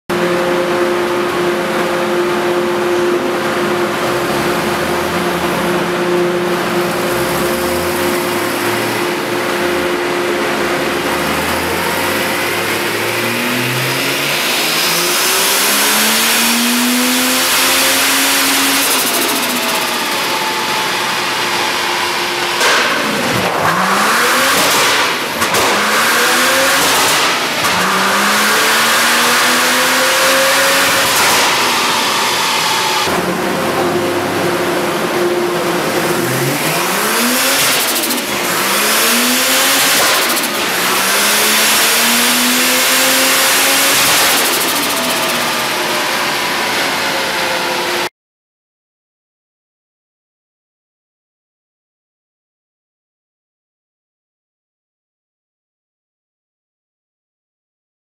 dyno tuning - haltech ps500 + 1fz turbo - 6.8 PSI
موتر طارق العقيل
pro car garage

تصوير موسي الفهيد